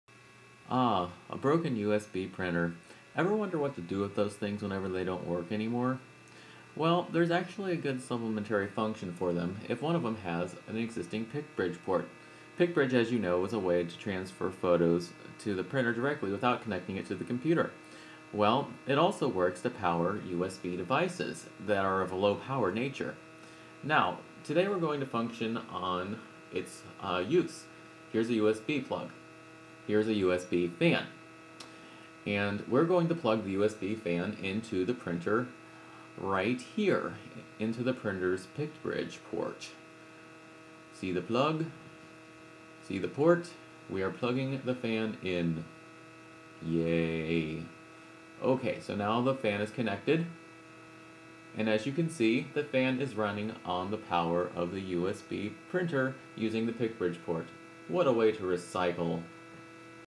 PictBridge Fan
Did you ever wonder to yourself, "is there anything that can be done with an old printer that doesn't work anymore?"   Well, if you did, or even if you didn't, this video gives one recollection of how to utilize an old printer that no longer functions as a printer.   If it has a PictBridge port, the port can be utilized as a USB power outlet.   I have used this printer to charge my iPod and run this USB fan.   Can you think of another clever way to use this?